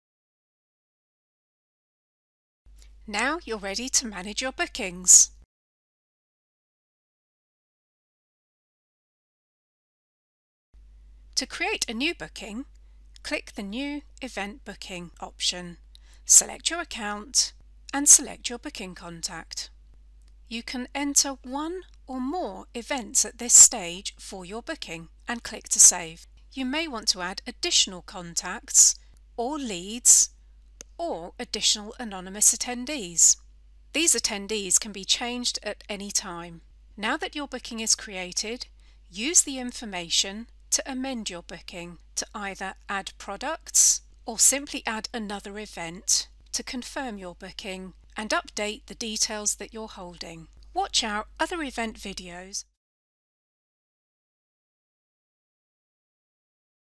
How to manage event bookings
Discover how to manage your event bookings, including how to create new bookings, in this quick video